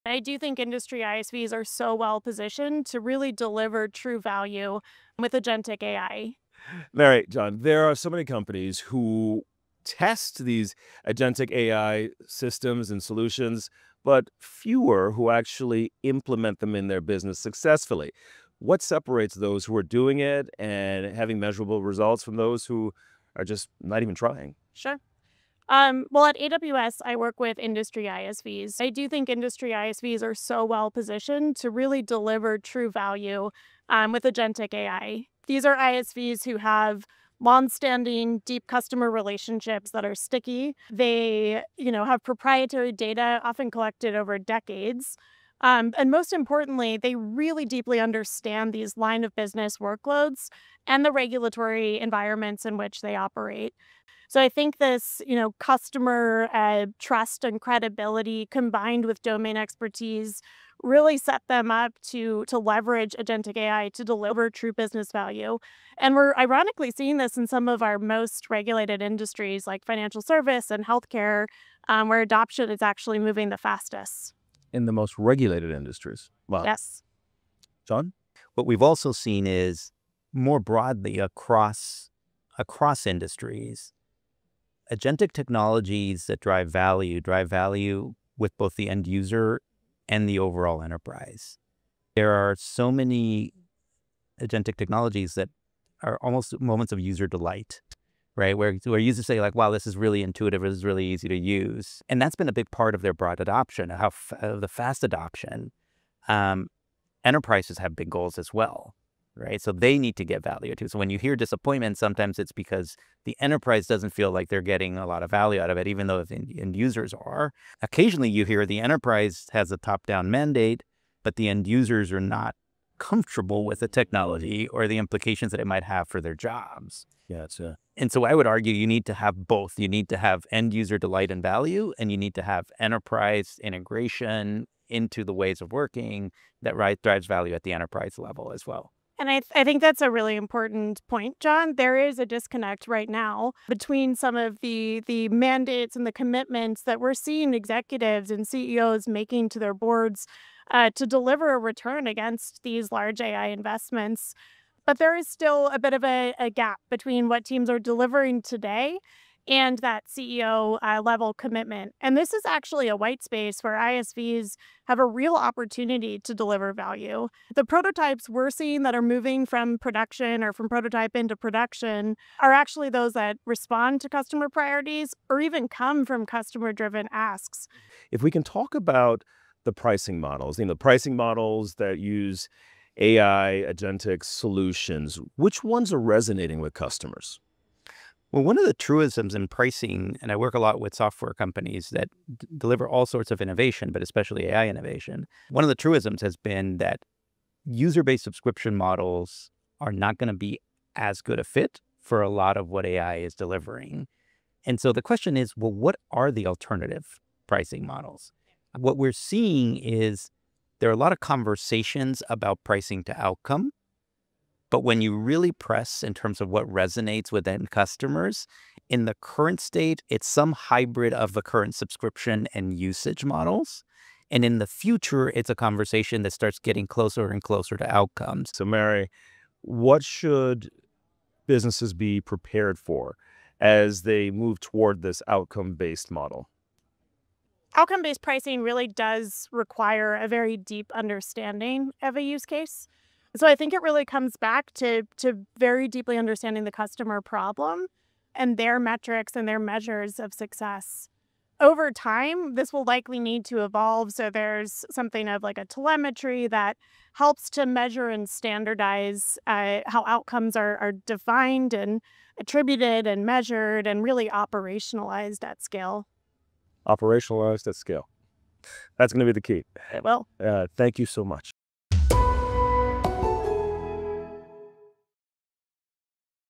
How Software Companies Are Putting Agentic AI to Work
Mary Palmer of AWS and BCG’s John Pineda explain how software providers pair domain expertise and customer-led use cases to deliver meaningful value with agentic AI.